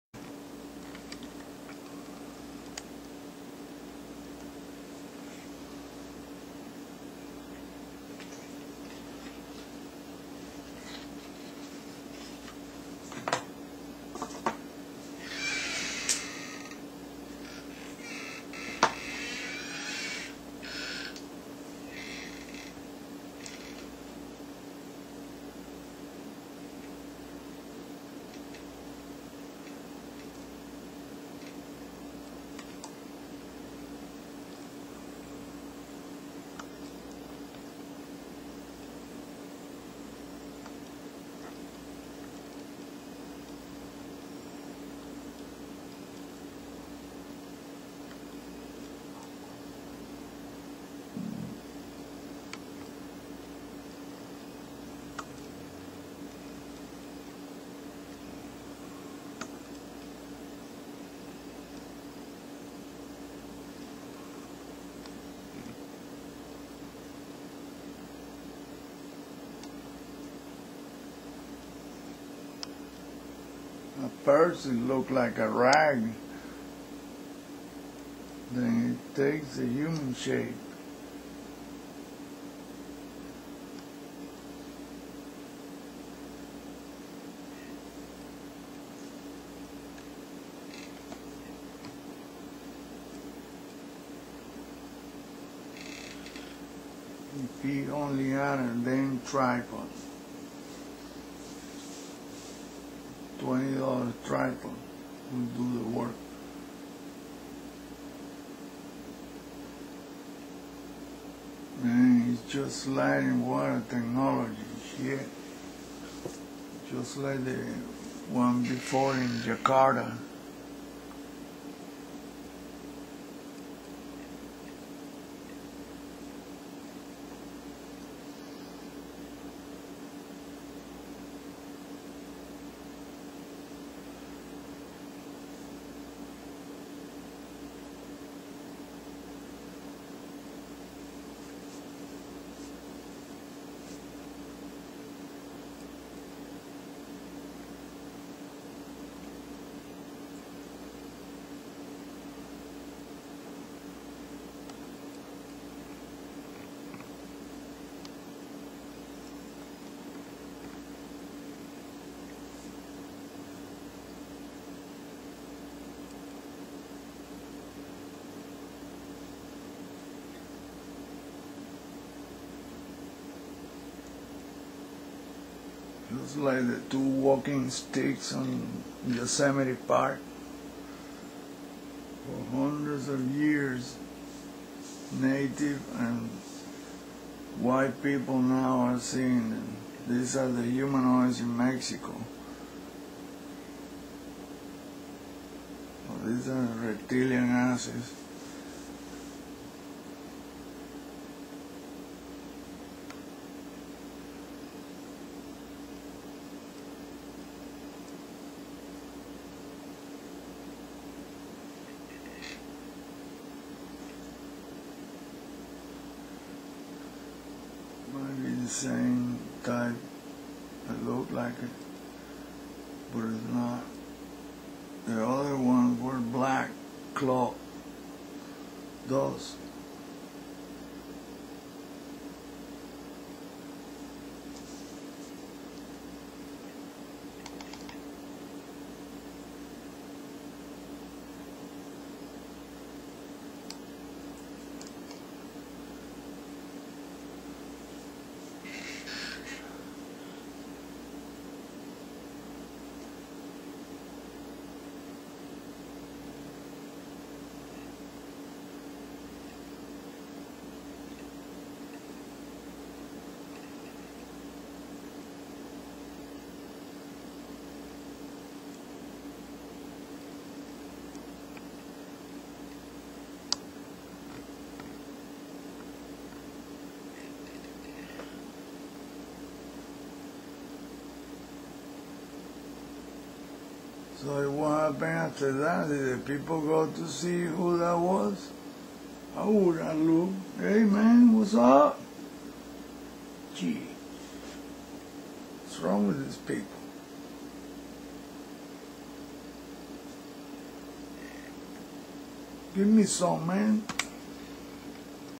JAPAN's FLYING BEING
a BEING DESCENDING FROM THE SKY WAS RECORDED IN WAKAYAMA, JAPAN. aLL OVER THE WORLD PEOLE ARE STARTING TO SEE WHAT JUST TO BE LEGENDS OF FOLKLORE, THANKS TO VIDEO CAMERAS